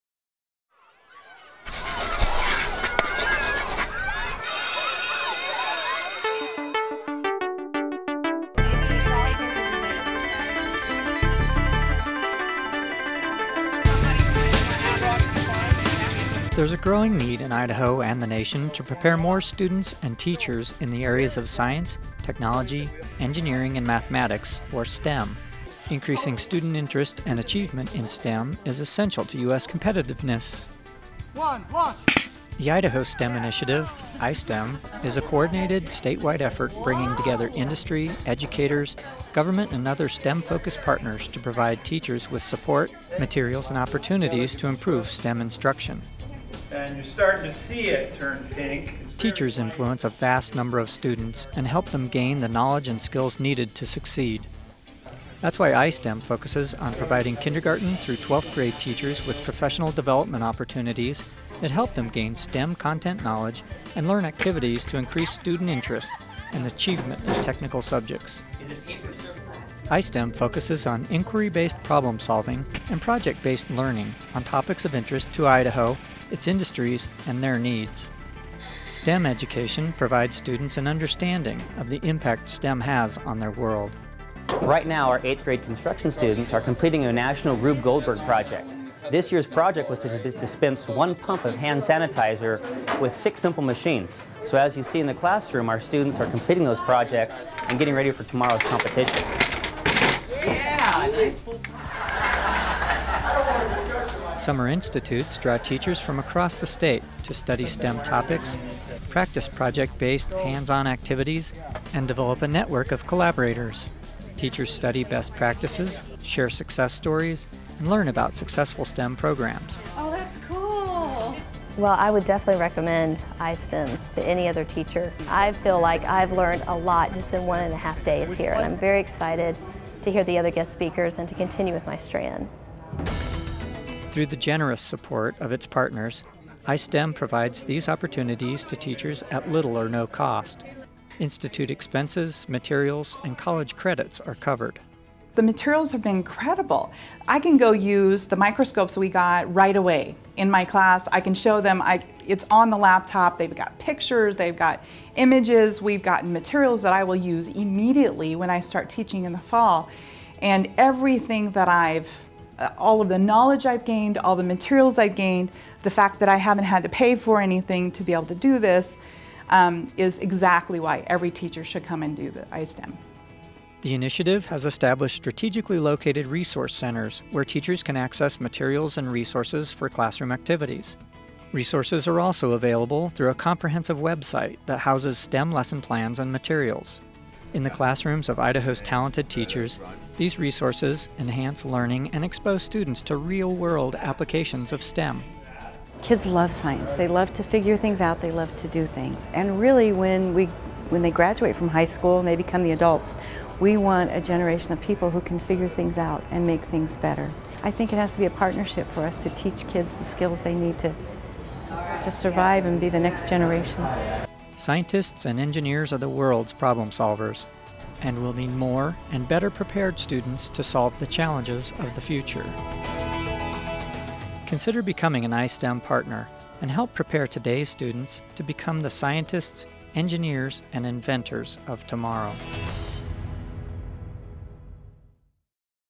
Idaho Science, Technology, Engineering and Mathematics Overview
Idaho National Laboratory has been instrumental in establishing the Idaho Science, Technology, Engineering and Mathematics initiative -- i-STEM, which brings together industry, educators, government and other partners to provide K-12 teachers with support, materials and opportunities to improve STEM instruction and increase student interest in technical careers.